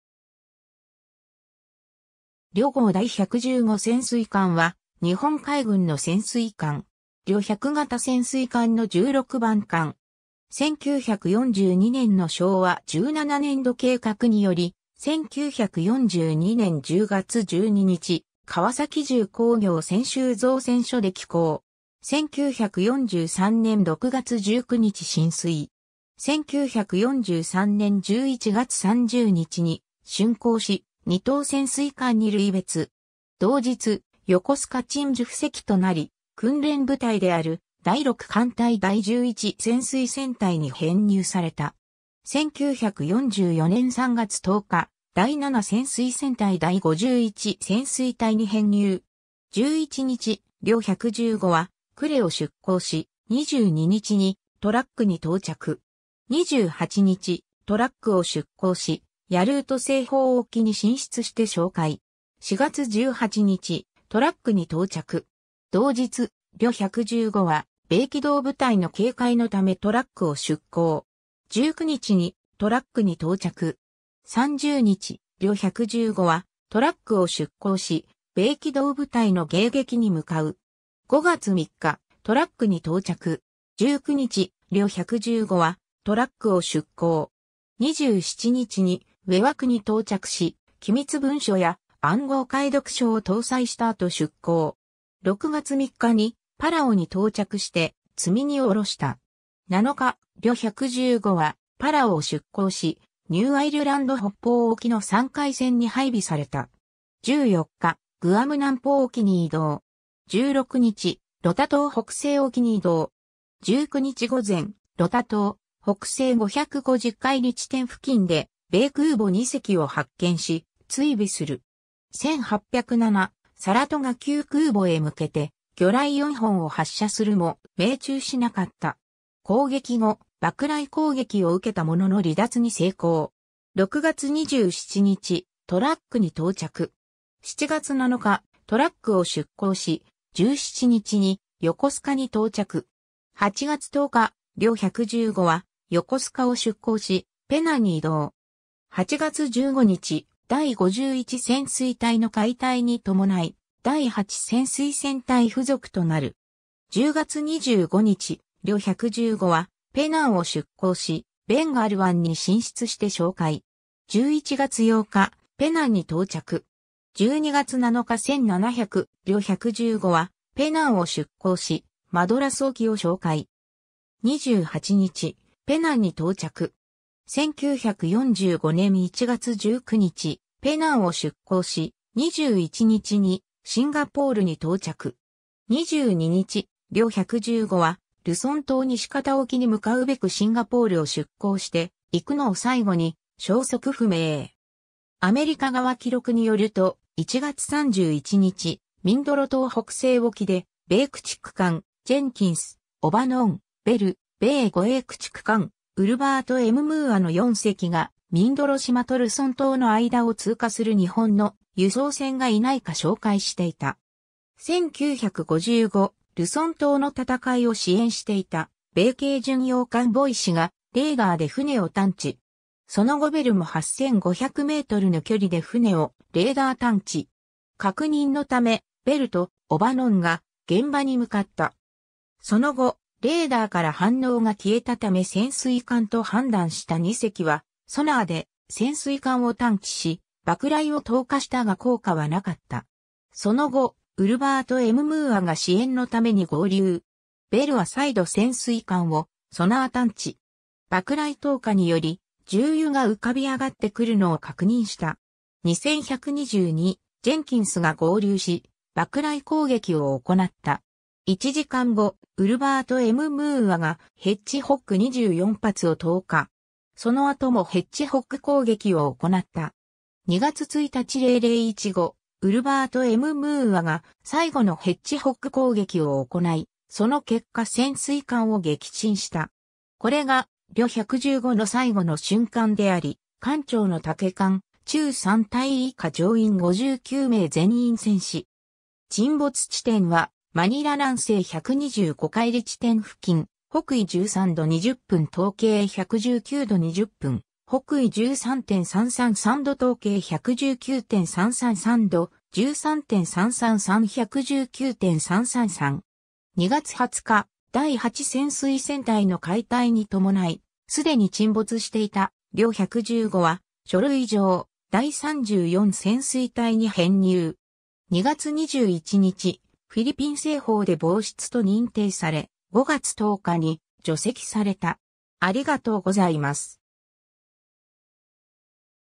呂号第百十五潜水艦
呂号第百十五潜水艦（ろごうだいひゃくじゅうごせんすいかん）は、日本海軍の潜水艦。
呂百型潜水艦（小型）の16番艦。
 1942年（昭和17年）の昭和17年度計画（マル急計画）により、1942年10月12日、川崎重工業泉州造船所で起工。
1943年（昭和18年）6月19日進水。
1943年11月30日に竣工し、二等潜水艦に類別。
同日、横須賀鎮守府籍となり、訓練部隊である第六艦隊第11潜水戦隊に編入された。
 1944年（昭和19年）3月10日、第7潜水戦隊第51潜水隊に編入。
 11日、呂115は呉を出港し、22日にトラックに到着。
28日、トラックを出港し、ヤルート西方沖に進出して哨戒。
4月18日、トラックに到着。
 同日、呂115は米機動部隊の警戒のためトラックを出港。
19日にトラックに到着。
 30日、呂115はトラックを出港し、米機動部隊の迎撃に向かう。
5月3日、トラックに到着。
 19日、呂115はトラックを出港。
27日にウェワクに到着し、機密文書や暗号解読書を搭載した後出港。
6月3日にパラオに到着して積荷を降ろした。
 7日、呂115はパラオを出港し、ニューアイルランド北方沖の散開線に配備された。
14日、グアム南方沖に移動。
16日、ロタ島北西沖に移動。
19日午前、ロタ島北西550浬地点付近で米空母2隻を発見し、追尾する。
1807、サラトガ級空母へ向けて魚雷4本を発射するも、命中しなかった。
攻撃後、爆雷攻撃を受けたものの離脱に成功。
6月27日、トラックに到着。
7月7日、トラックを出港し、17日に横須賀に到着。
 8月10日、呂115は横須賀を出港し、ペナンに移動。
8月15日、第51潜水隊の解隊に伴い、第8潜水戦隊付属となる。
 10月25日、呂115はペナンを出港し、ベンガル湾に進出して哨戒。
11月8日、ペナンに到着。
 12月7日1700、呂115はペナンを出港し、マドラス沖を哨戒。
28日、ペナンに到着。
1945年（昭和20年）1月19日、ペナンを出港し、21日にシンガポールに到着。
 22日、呂115はルソン島西方沖に向かうべくシンガポールを出港していくのを最後に消息不明。
 アメリカ側記録によると、1月31日、ミンドロ島北西沖で米駆逐艦ジェンキンス(USS Jenkins, DD-447)、オバノン(USS O'Bannon, DD-450)、ベル(USS Bell, DD-587)、米護衛駆逐艦ウルヴァート・M・ムーア(USS Ulvert M. Moore, DE-442)の4隻がミンドロ島とルソン島の間を通過する日本の輸送船がいないか哨戒していた。
1955、ルソン島の戦いを支援していた米軽巡洋艦ボイシ(USS Boise, CL-47)がレーダーで船を探知。
その後ベルも8500mの距離で船をレーダー探知。
確認のためベルとオバノンが現場に向かった。
その後、レーダーから反応が消えたため潜水艦と判断した2隻はソナーで潜水艦を探知し、爆雷を投下したが効果はなかった。
その後、ウルヴァート・M・ムーアが支援のために合流。
ベルは再度潜水艦をソナー探知。
爆雷投下により、重油が浮かび上がってくるのを確認した。
2122、ジェンキンスが合流し、爆雷攻撃を行った。
1時間後、ウルヴァート・M・ムーアがヘッジホッグ24発を投下。
その後もヘッジホッグ攻撃を行った。
2月1日0015、ウルヴァート・M・ムーアが最後のヘッジホッグ攻撃を行い、その結果潜水艦を撃沈した。
これが呂115の最期の瞬間であり、艦長の竹間忠三大尉以下乗員59名全員戦死。
沈没地点はマニラ南西125浬地点付近、 北緯13度20分 東経119度20分 / 北緯13.333度 東経119.333度 / 13.333; 119.333。
 2月20日、第8潜水戦隊の解隊に伴い、既に沈没していた呂115は書類上第34潜水隊に編入。
 2月21日、フィリピン西方で亡失と認定され、5月10日に除籍された。